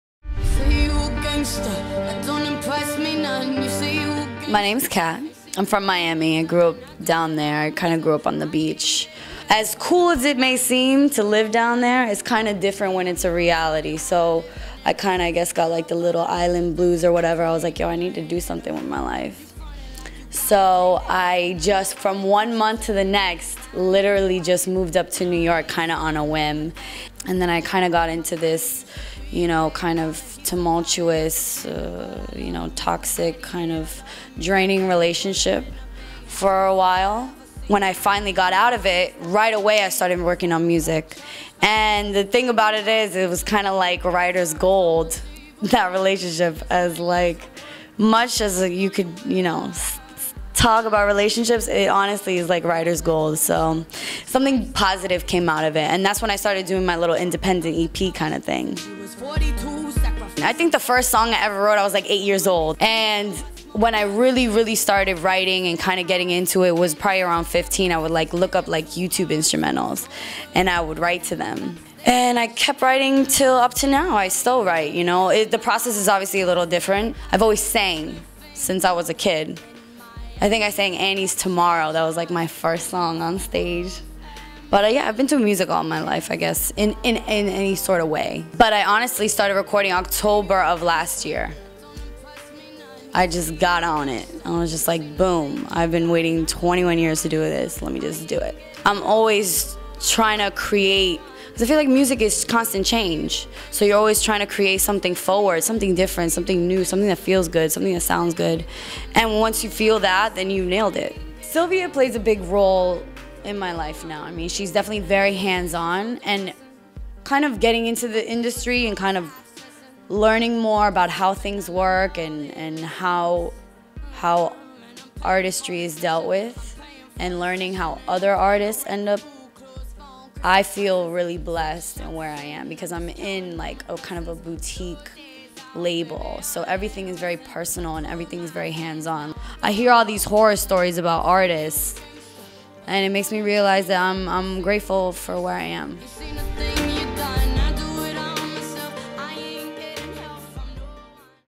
Kat Dahlia Talks Musical Beginnings and Working With Sylvia Rhone
Epic Records signee Kat Dahlia discusses writing music from a young age and how music industry exec Sylvia Rhone plays a big role in her career.